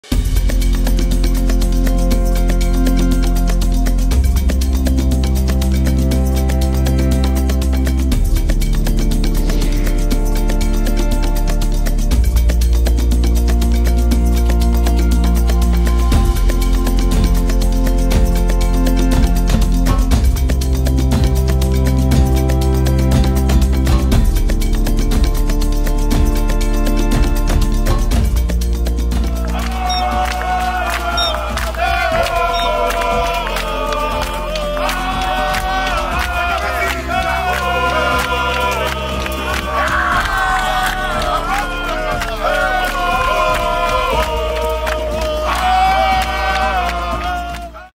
Grand Traditional Welcome of Prime Minister Narendra Modi in Johannesburg
Prime Minister Narendra Modi receives a traditional welcome and is accorded a ceremonial 'Guard of Honour' as he arrives in Johannesburg, South Africa, to attend the 15th BRICS Summit meeting.